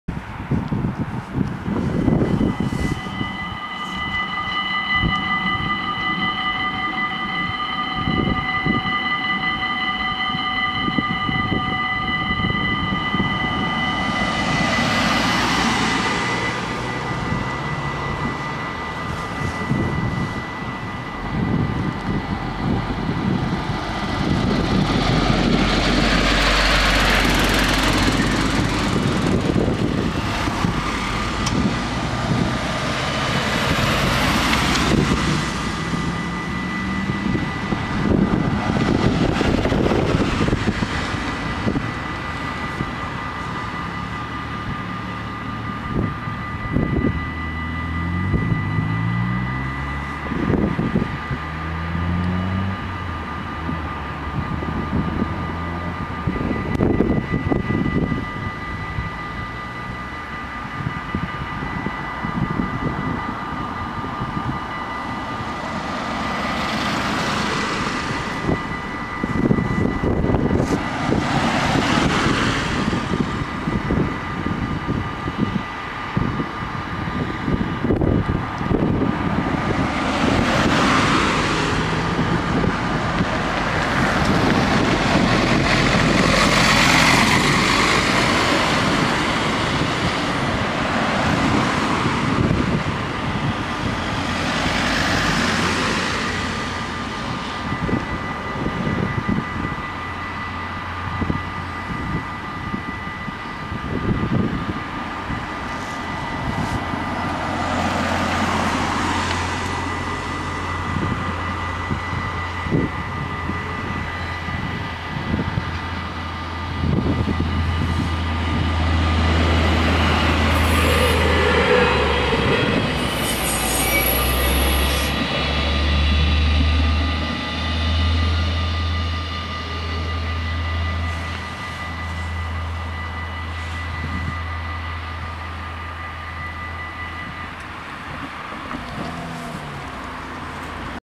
Spoorwegovergang Appingedam // Dutch railroad crossing
Locatie: Fivelweg
Traject: Delfzijl - Groningen
Soort: AHOB
Rode lichten: 19
Oranje pijl: 2
Bellen: 5
Bomen: 4
Andreaskruisen: 4
Passeren:
- GTW-D als ST Groningen → Delfzijl